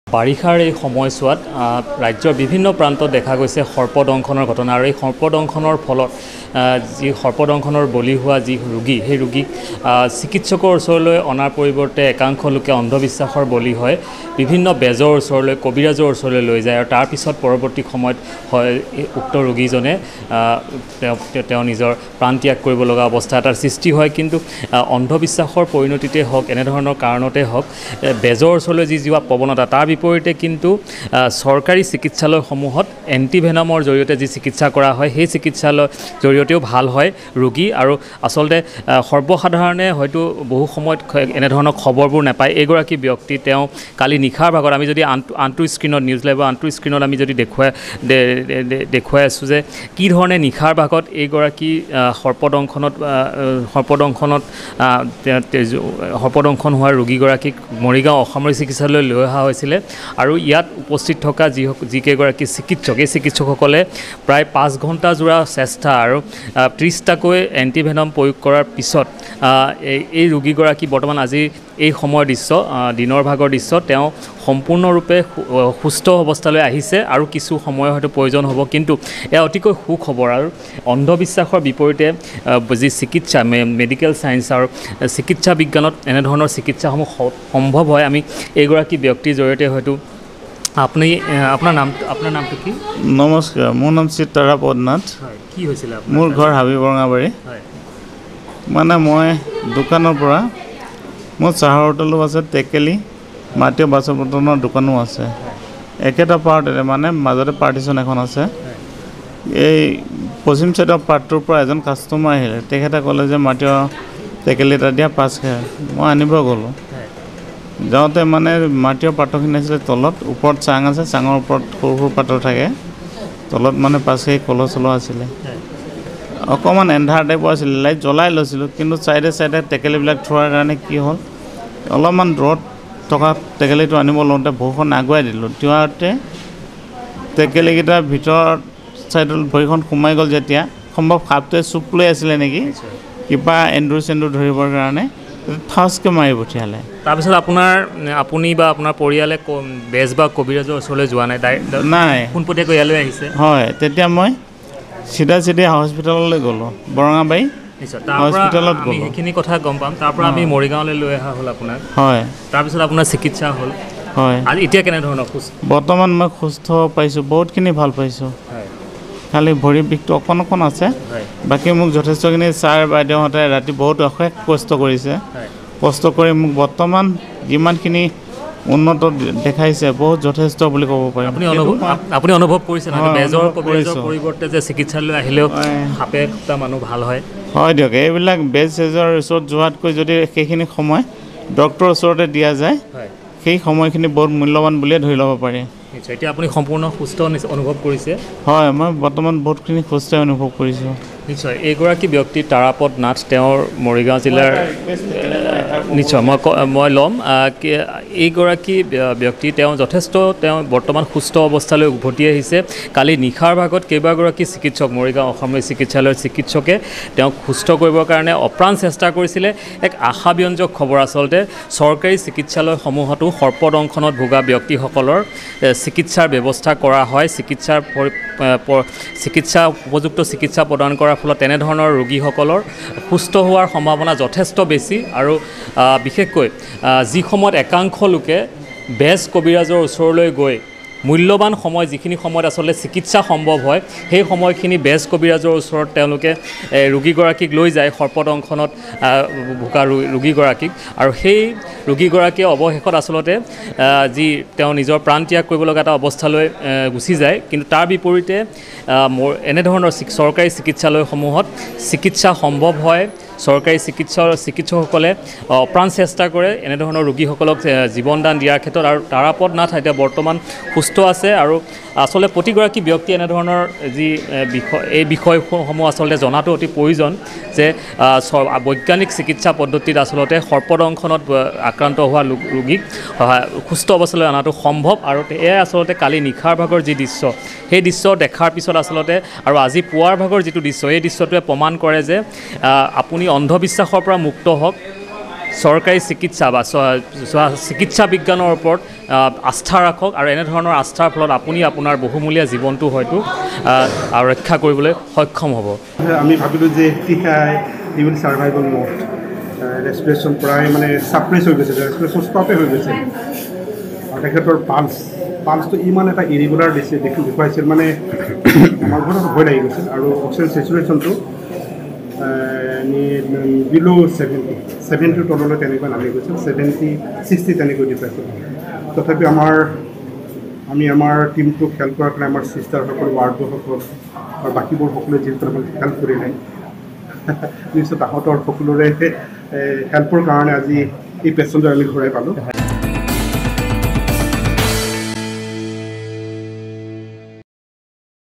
ফেঁটী সাপে দংশন কৰা লোকক মৃত্য়ুৰ মুখৰ পৰা ঘূৰাই আনিলে চৰকাৰী চিকিৎসকে
🔴 মৰিগাঁৱৰ তাৰাপদ নাথক নতুন জীৱন দিলে চৰকাৰী হাস্পতালৰ চিকিৎসকৰ দলে